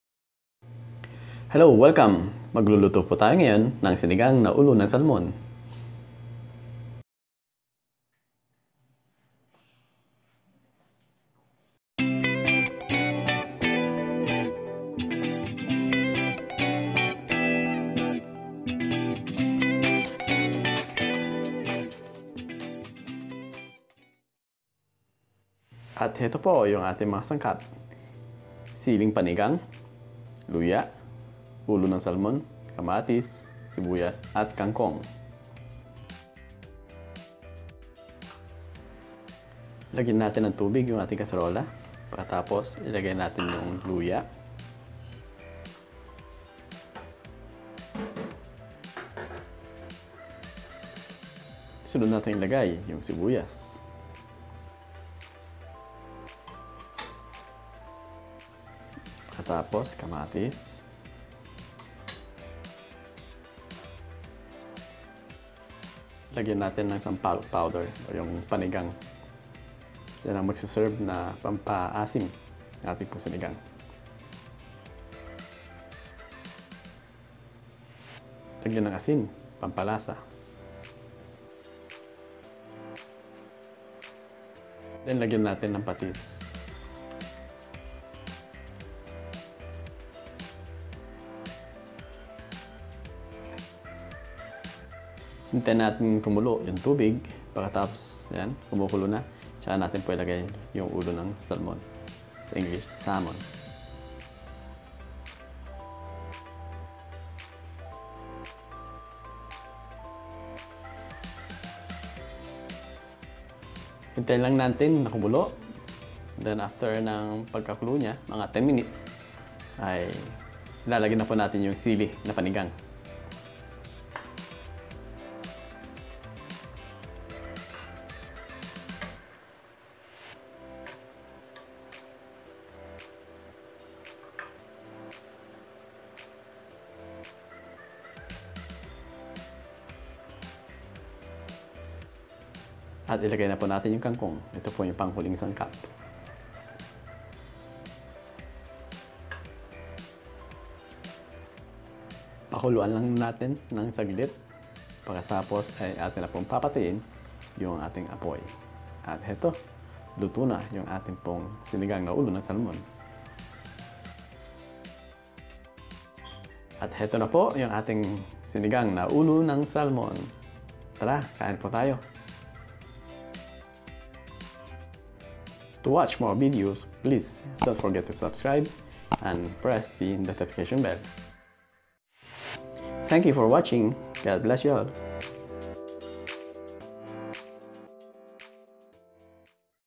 Sinigang Na Ulo Ng Salmon
Itong recipe na ito ay mayaman sa Omega 3 at fiber na makakabuti sa ating mga puso.

Hope this video helps you on how to cook sinigang na ulo ng salmon.